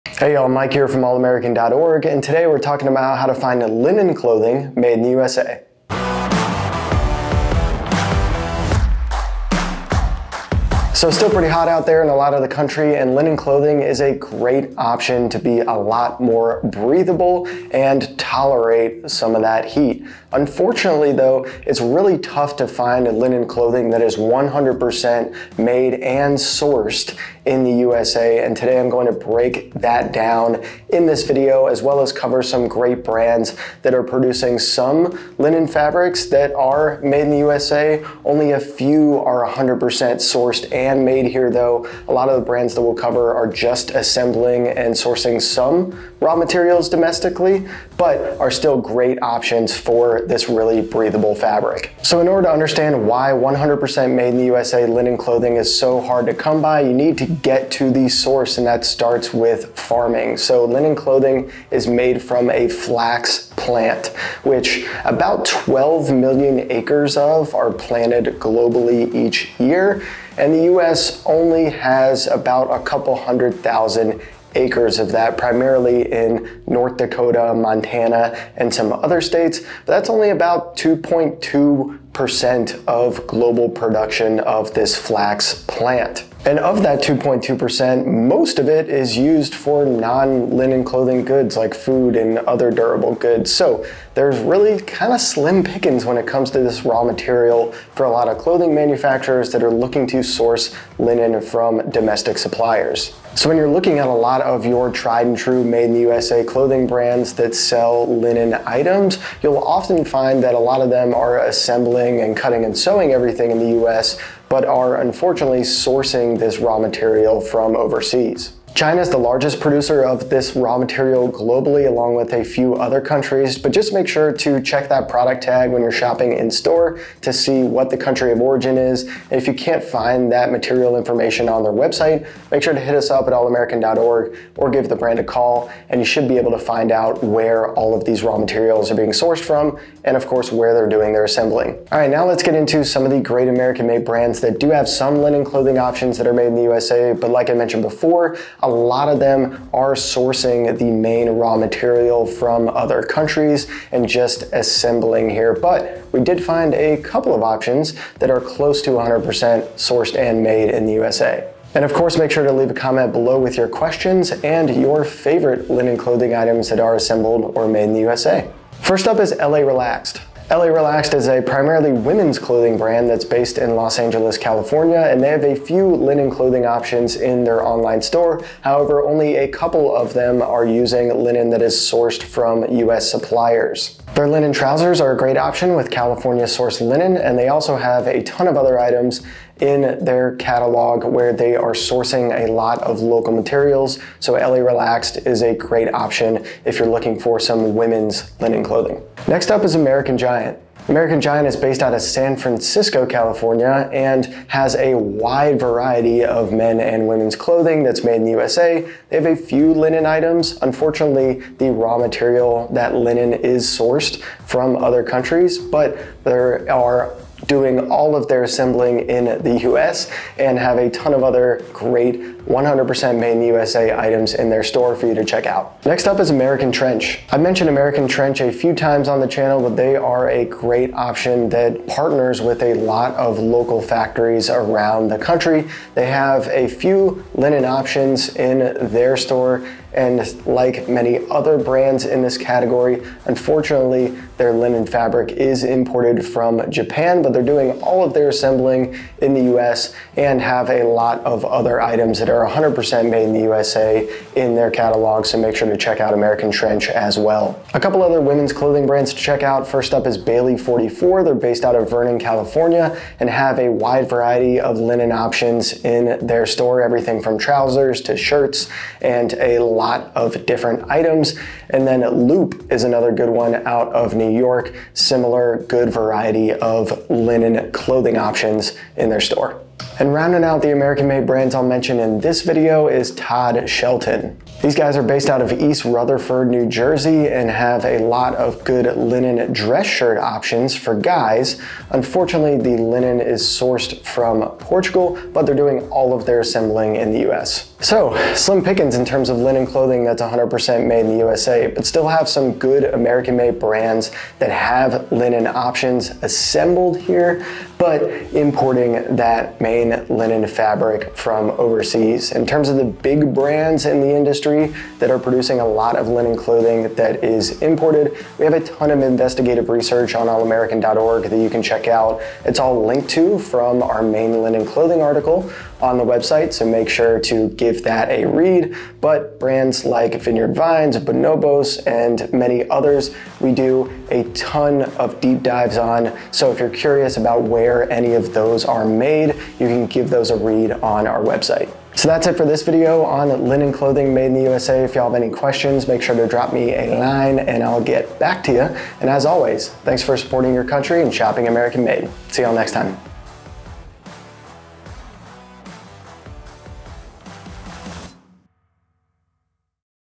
Linen Clothing Made in the USA: Any Good Options?
In this video, we're talking about linen clothing made in the USA. This is a tough material to find US suppliers for, so it was difficult to find a lot of brands producing linen clothing 100% made in the USA with domestically sourced raw materials. But, we still found some great American made linen clothing brands!

Sections:
0:00 Intro
0:52 Material Sourcing and Manufacturing Notes
2:22 Linen Clothing Made in the USA
4:58 Where the Big Brands Are Made